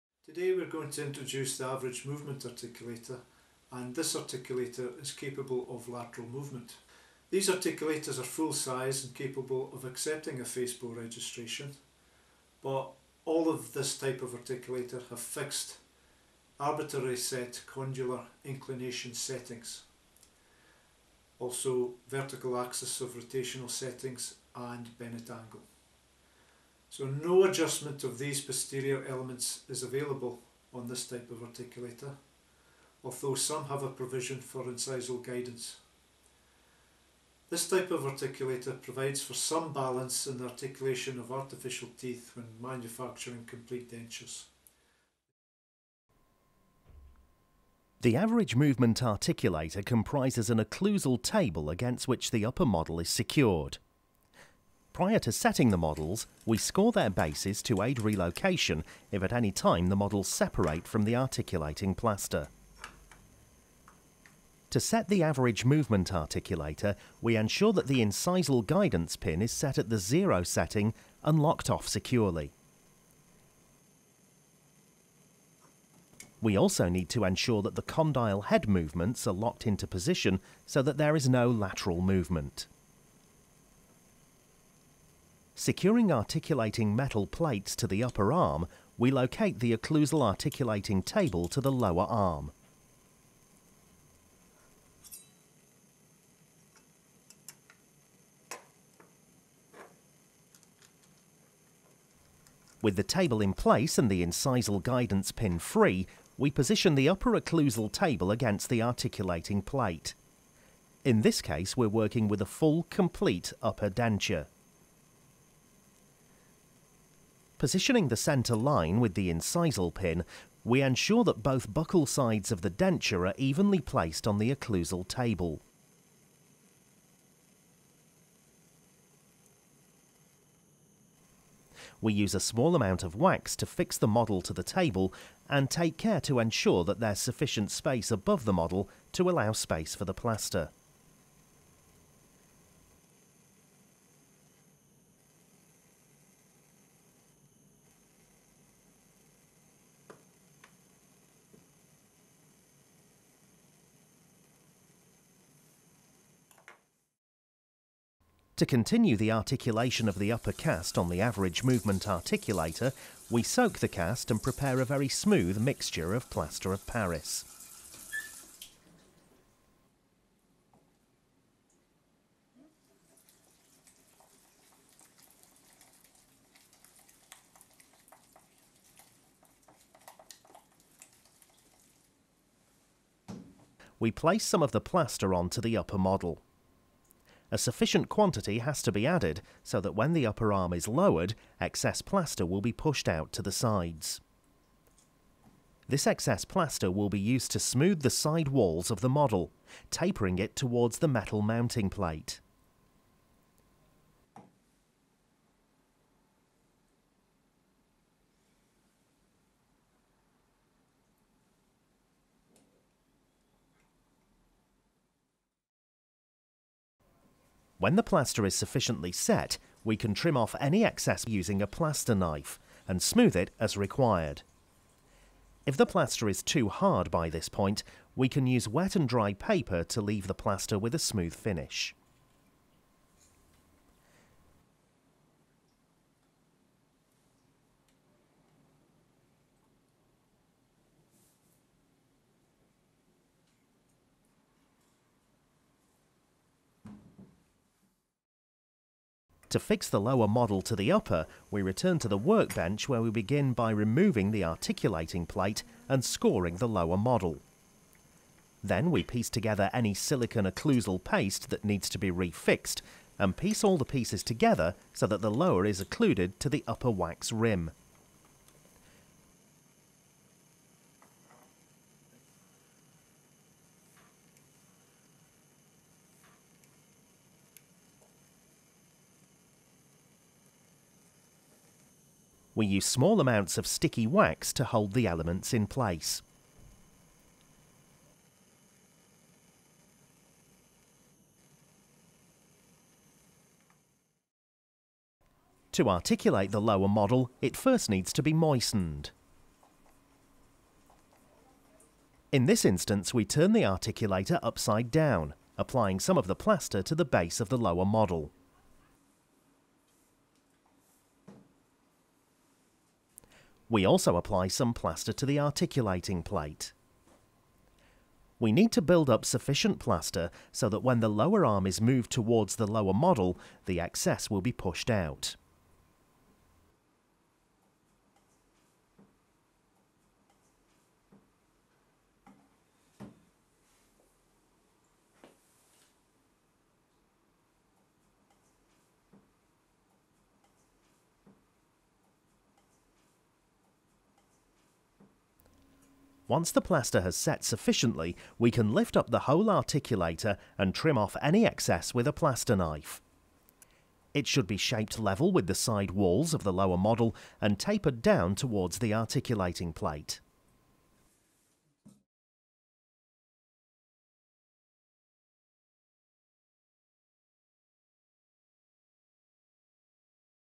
Average value articulation
A dental laboratory technique to mount casts / models on an average movement articulator.